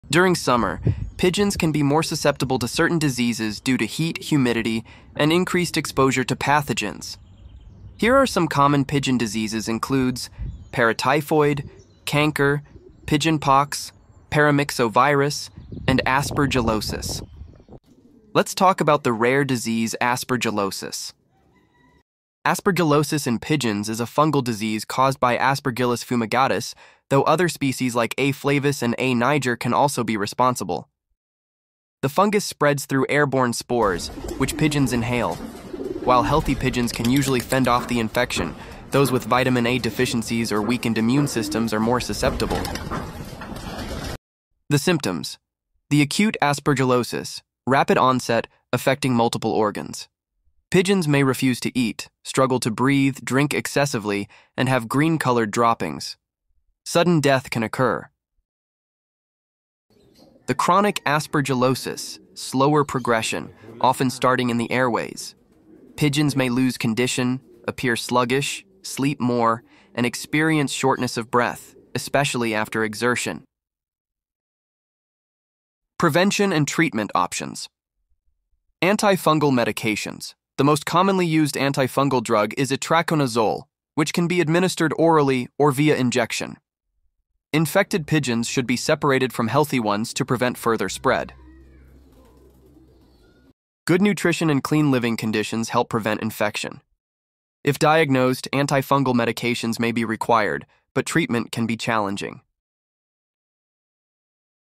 The Rare Pigeon Disease Aspergillos - Aspergillus fumigatus
During summer, pigeons can be more susceptible to certain diseases due to heat, humidity, and increased exposure to pathogens. here are some common pigeon diseases includes,

Paratyphoid, Canker, Pigeon Pox, Paramyxovirus, and Aspergillosis.